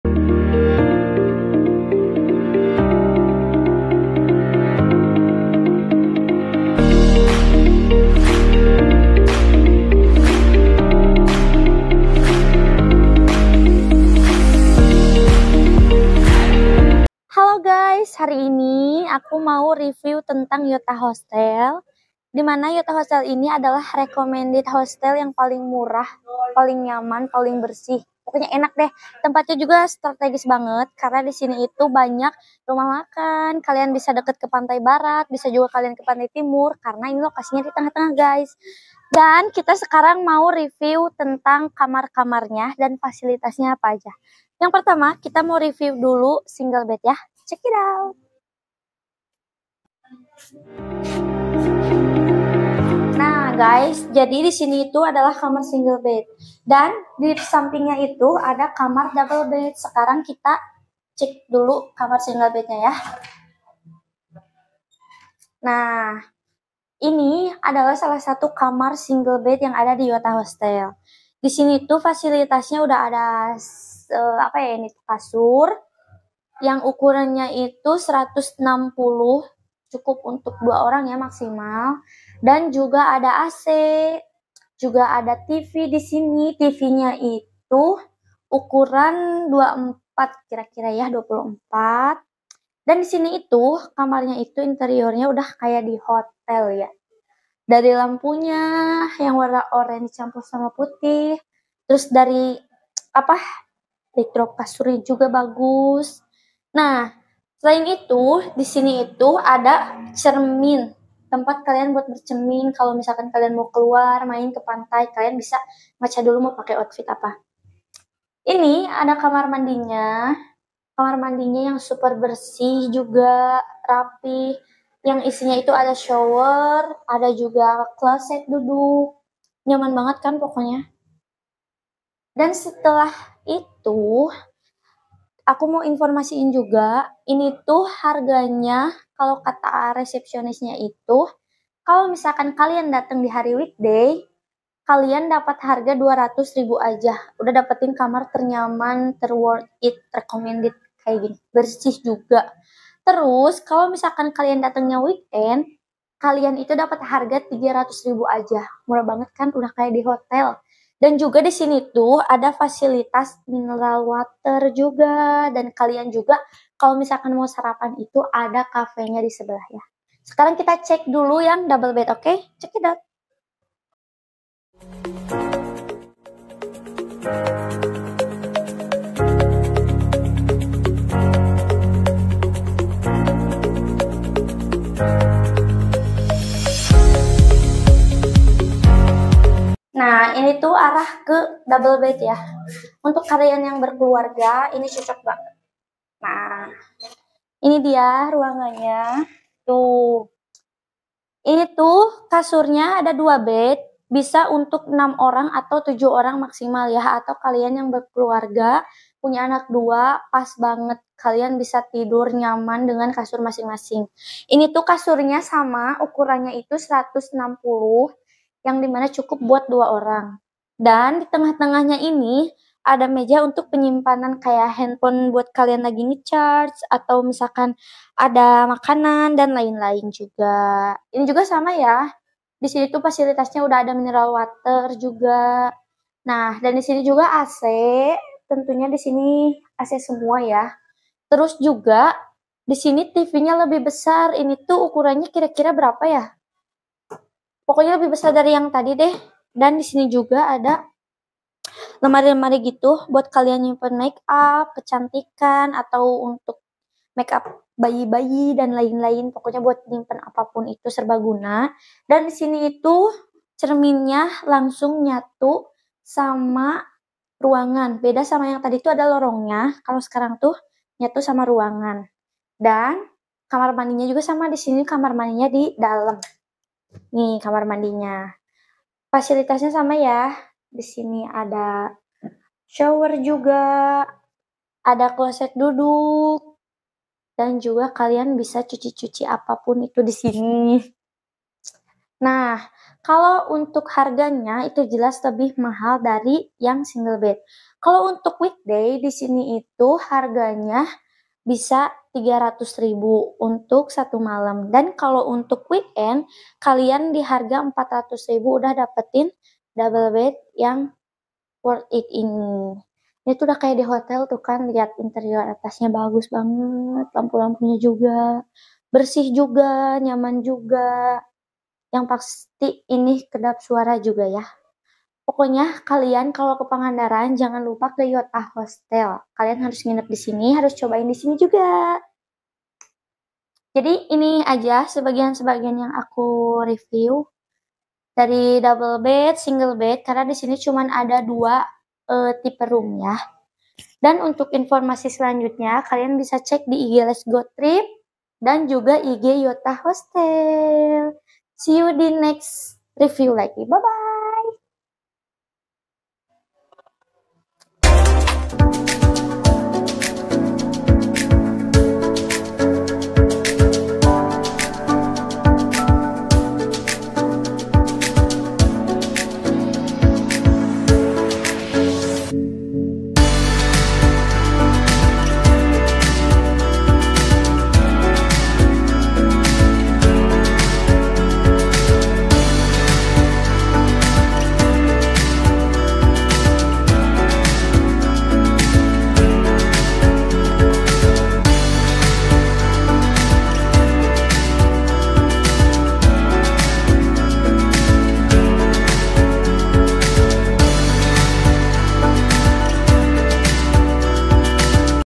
🔴PENGINAPAN DI PANGANDARAN ADA YANG SE'BAGUS INI TAPI MURAH✅
Hallo kembali lagi bersama kami Letsgotrip19

Kali ini kita nge review salahsatu Hostel //penginapan yang REKOMENDED banget menurut kita,
Fasilitas nya yg ga kalah sama hotel gede tapi harga bersahabat buat wisatawan,selain tempat ny yg nyaman,parkir NY juga luas banget.
Yukk ke Pangandaran dan simak semua hasil review kami agar kalian dapat informasi yang ada disini..